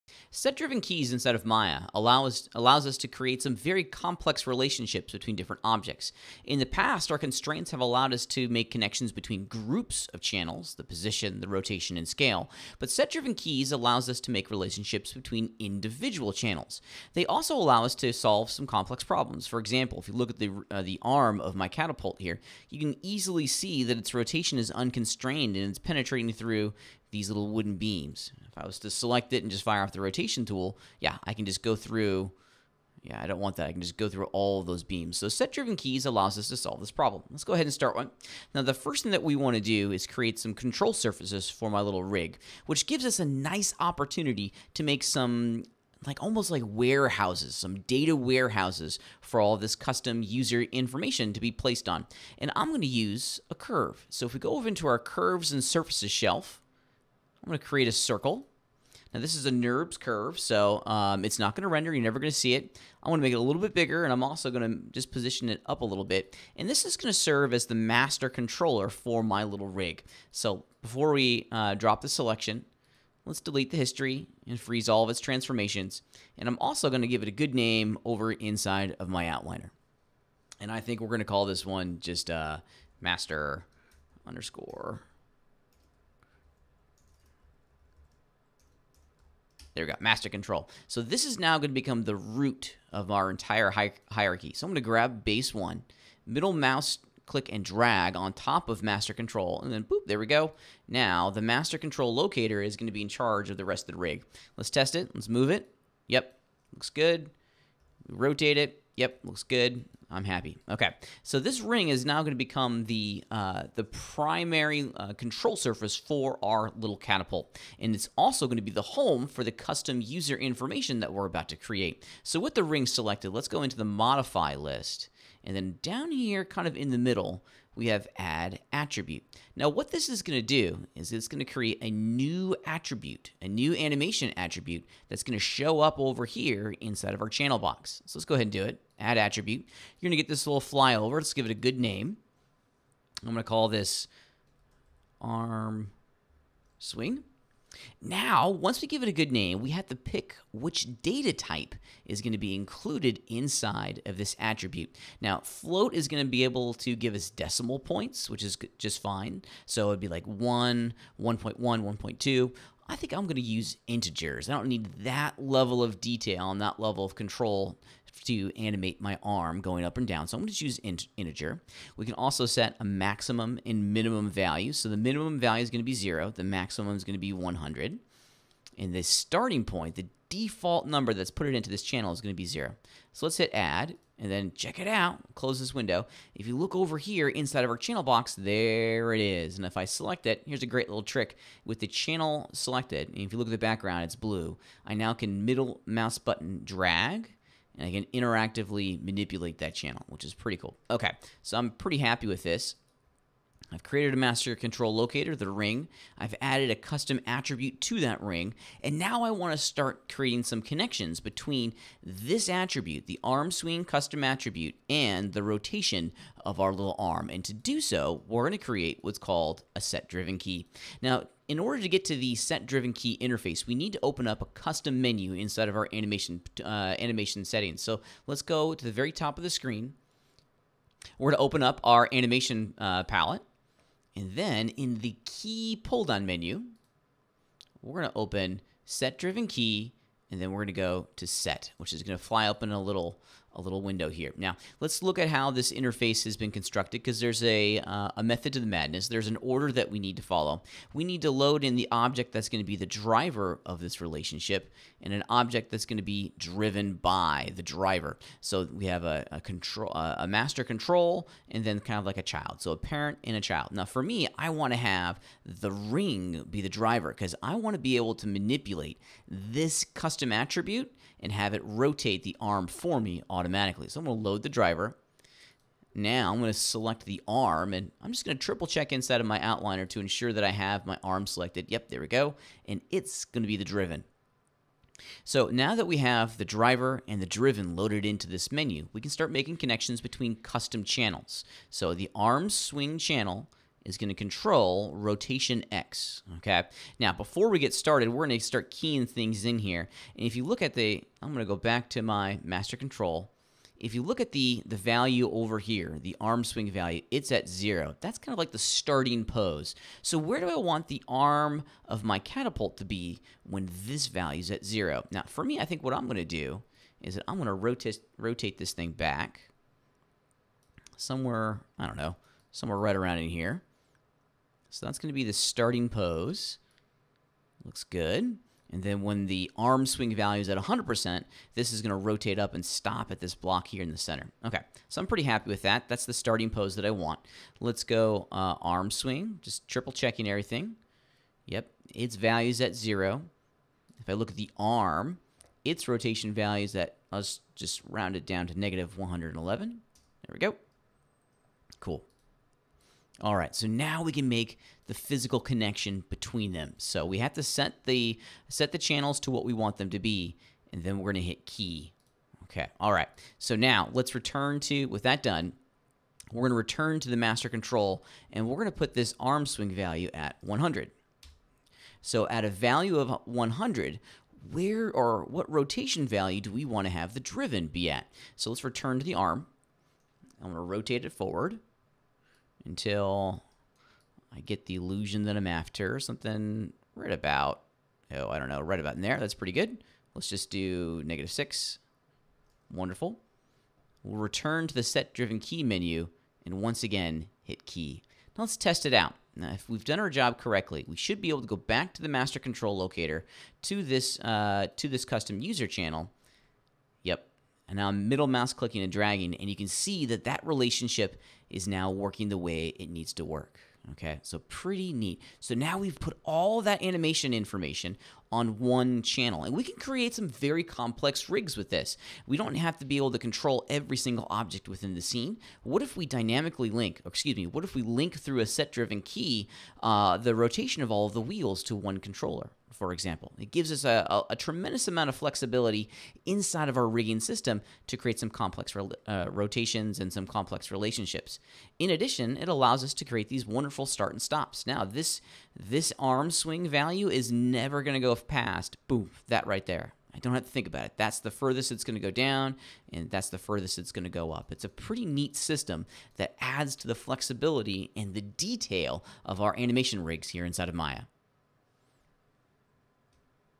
Set Driven Keyes
Creating complex relationships between channels in Maya is easy with Set Driven Keyes!